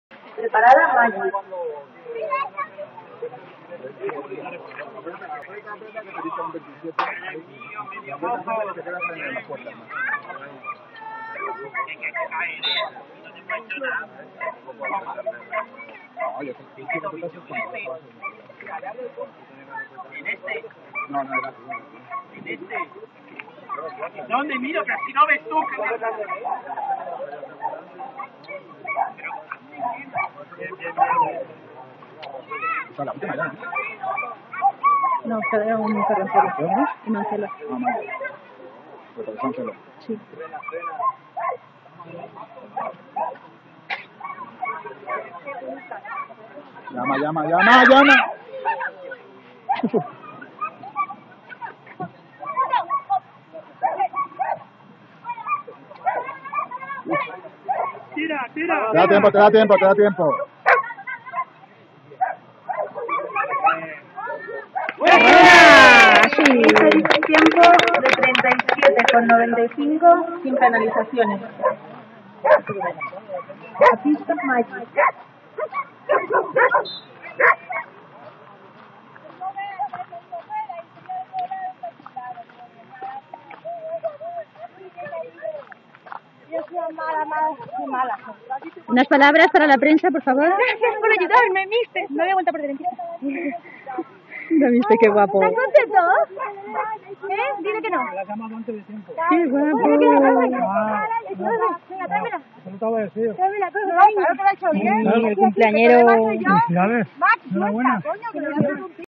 Angelica y Mister - Jumping Eslon - 1 Septiembre
Pese a perderse una vez y media en pista se han marcado el jumping a cero que necesitaban para el punto a cero! Eventualmente han quedado primeros aunque no es lo importante. GRAAAAAANDESSSSSS!